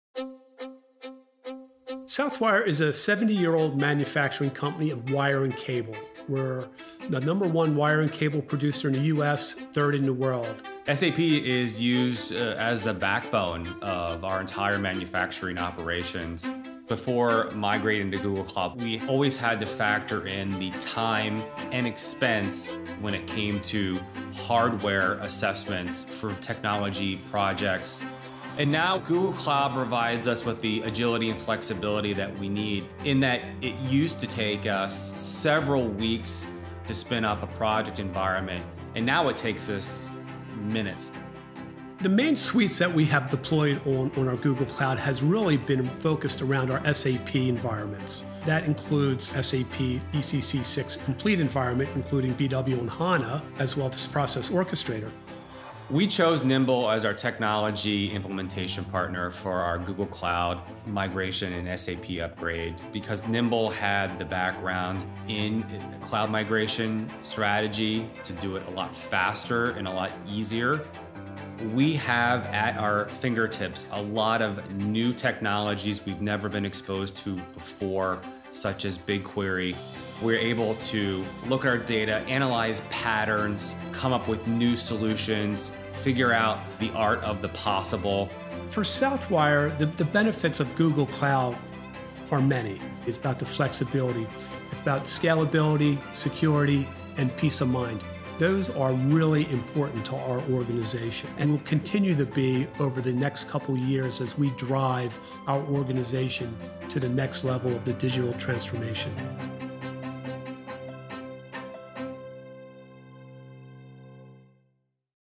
Southwire eliminates downtime to keep 30+ factories running 24/7
As one of the leading manufacturers of wire and cable used for transmission and distribution of electricity, Southwire aids the delivery of power to millions of people worldwide. They have more than 30 manufacturing facilities across the United States running 24/7. With help from Google Cloud and their implementation partner, NIMBL, Southwire completed the SAP migration to Google Cloud over a planned maintenance weekend on July 4th. Learn how the move of their SAP landscape to Google Cloud has helped Southwire eliminate downtime to keep 30+ factories running 24/7, protect them against ransomware attacks with improved cloud security as well as jumpstart their digital transformation initiatives including AI/ML capabilities